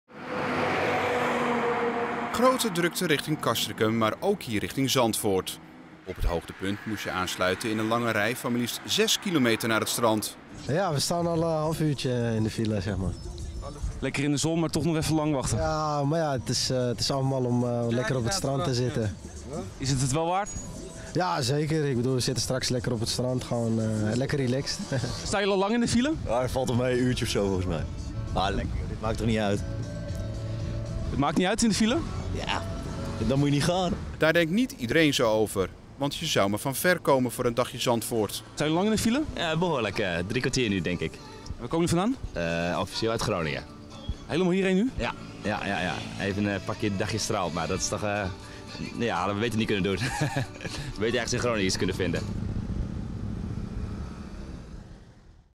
Lange files richting de stranden
Ondanks het risico dat de zeevonk ook Noord-Hollandse wateren zou teisteren, stonden er op deze tropische dag lange files richting de stranden.